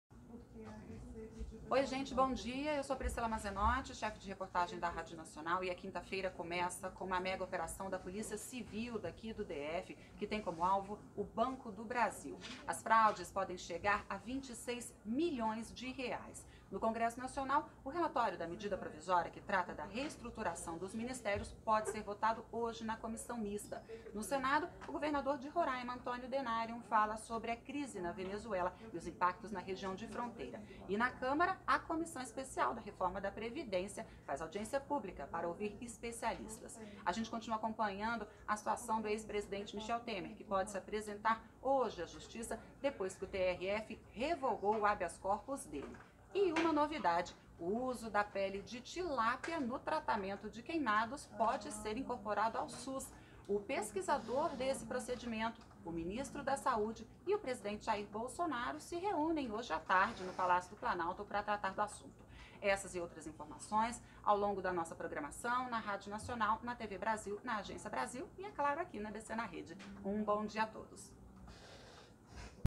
Minuto Brasil 09/05/2019
Operação da Polícia Civil do DF investiga fraudes no Banco do Brasil; relatório da MP da reestruturação de ministérios pode ser votado na Comissão Mista; e a possível apresentação de Michel Temer à Justiça. Acompanhe essas e outras notícias nos veículos EBC nesta quinta-feira (9).